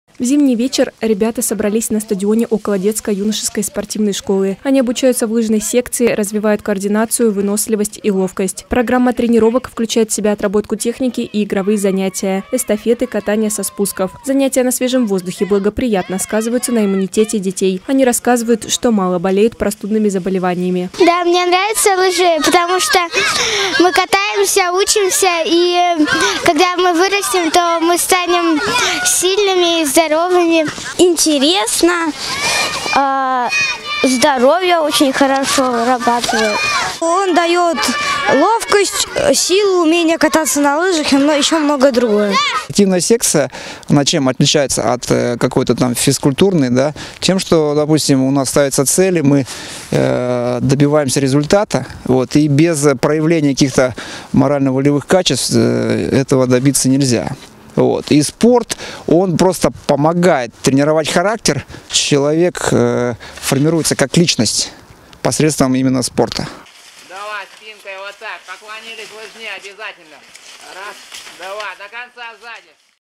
Десна-ТВ: Один день из жизни маленьких лыжников
Наша съемочная группа побывала на детской тренировке по лыжным гонкам. Как проходит тренировочный процесс  у юных спортсменов, смотрите в сюжете.